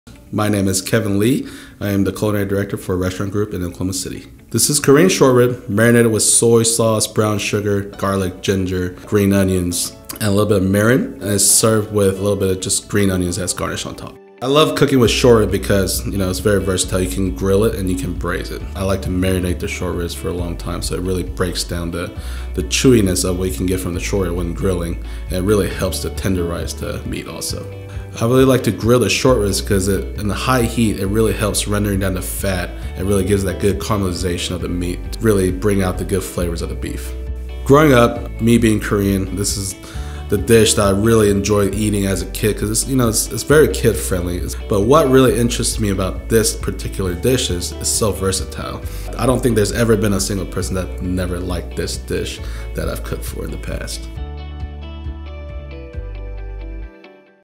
Korean Short Ribs featuring Oklahoma Beef
Chef Kevin Lee (@chefkleeokc) makes his childhood favorite Korean short ribs by marinating Oklahoma beef short ribs with soy sauce, brown sugar, garlic, ginger, green onions, mirin, and serving with green onions as garnish.